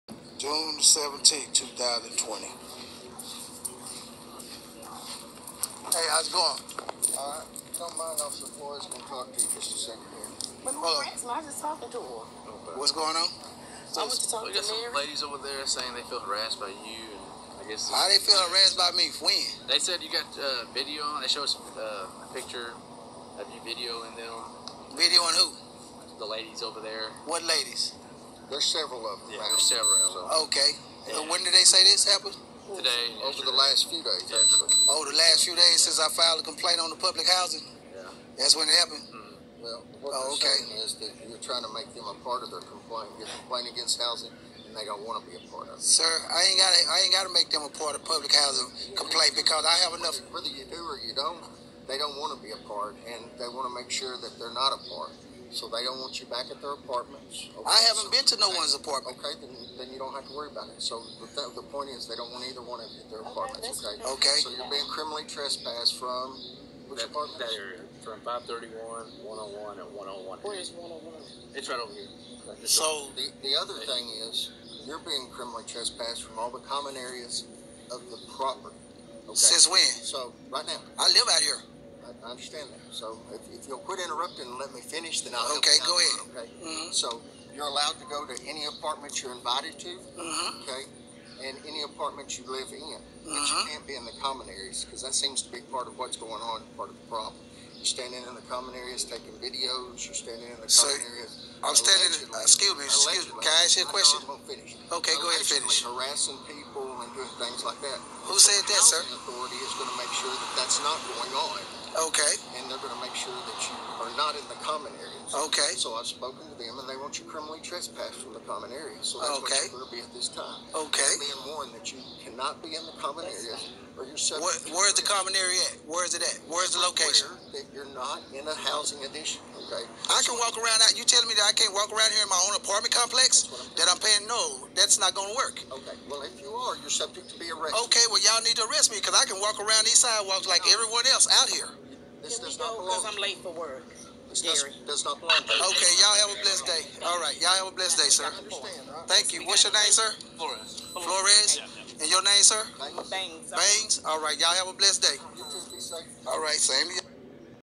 Man trespassed from Paris Housing Authority for filing complaints
A Paris Texas Man was trespassed from Paris Housing Authority after taking videos and photos of what he considered violations and then filing complaints. He was accused of harassment.